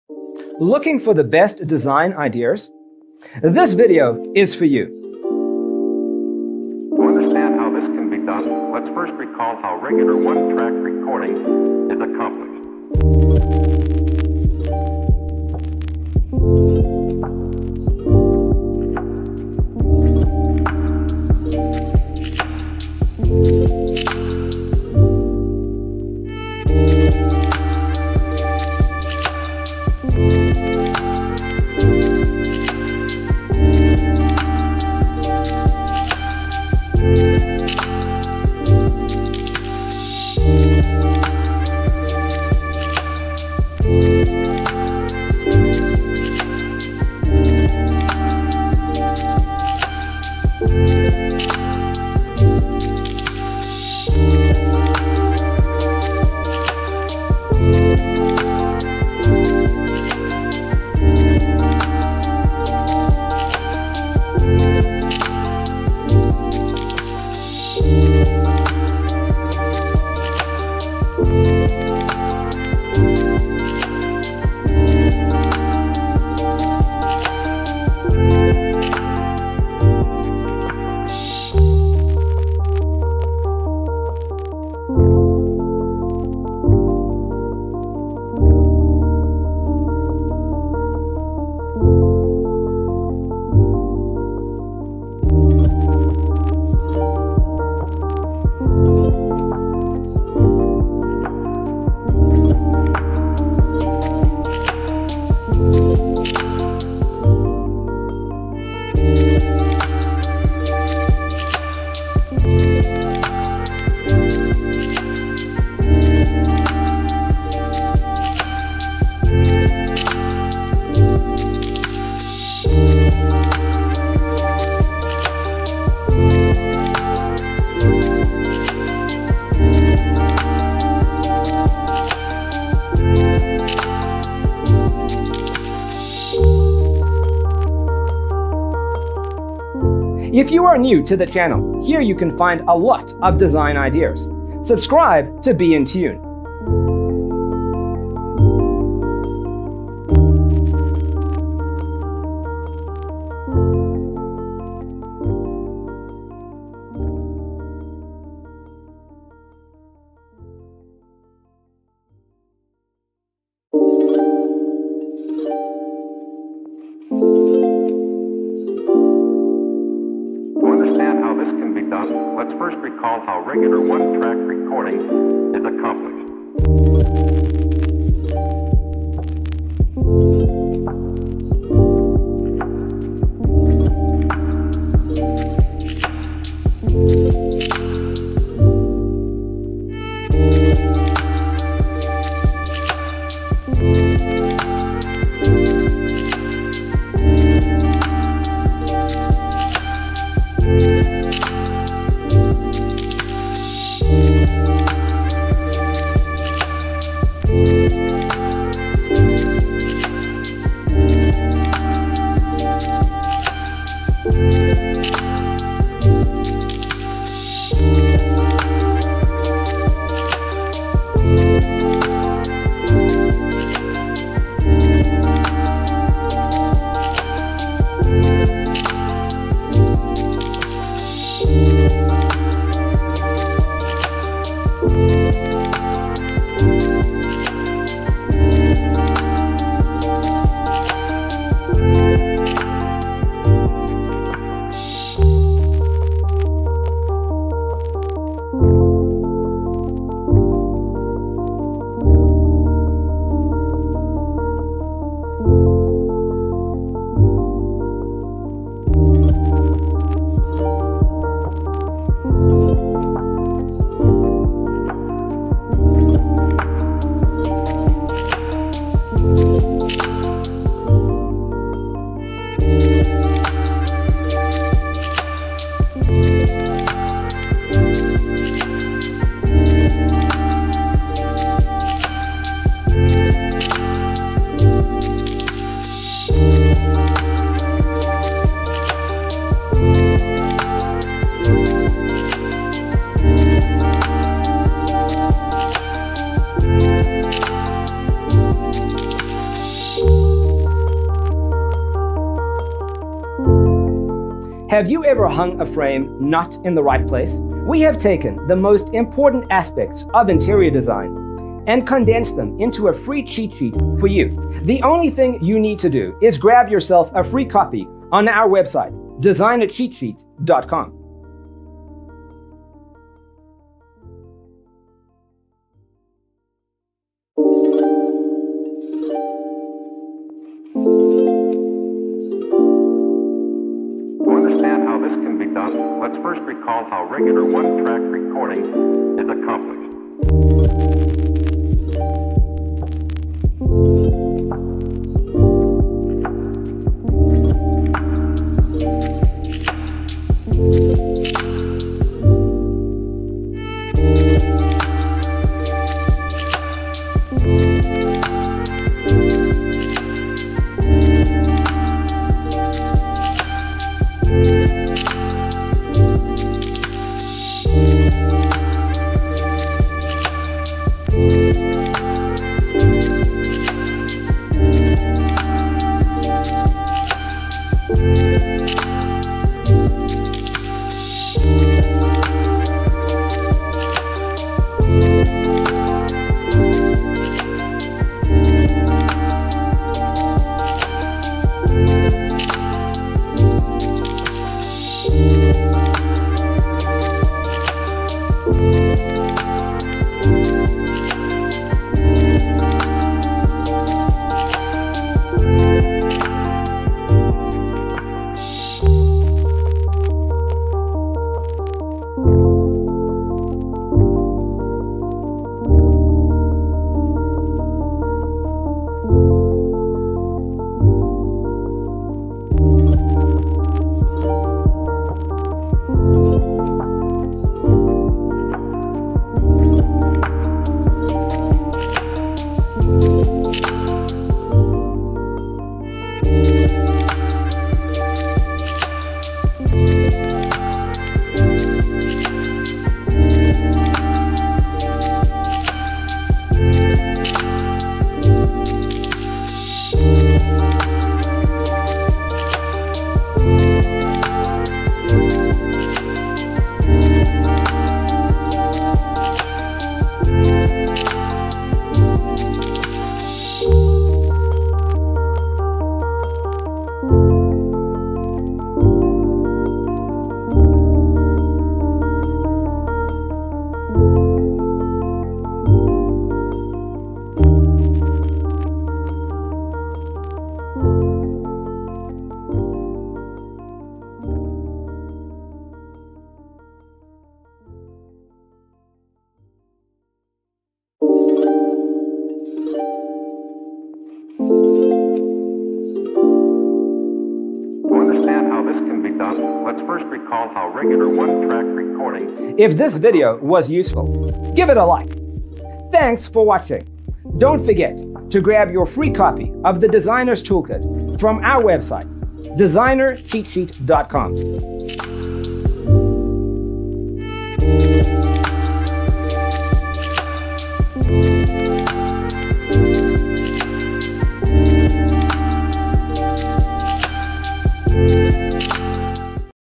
75 Craftsman Double-sink Bath Design Ideas You'll Love 🌈
Browse craftsman bathroom and powder room designs and decorating ideas. Discover inspiration for your arts and crafts bath remodel, including colors, storage, layouts and organization.

0:00:00 - Intro
0:00:12 - Craftsman Double-sink Bath Design Ideas You'll Love: Inspirations 1 to 25
0:03:03 - Craftsman Double-sink Bath Design Ideas You'll Love: Inspirations 26 to 50
0:06:07 - Craftsman Double-sink Bath Design Ideas You'll Love: Inspirations 51 to 75

Hello there!🌈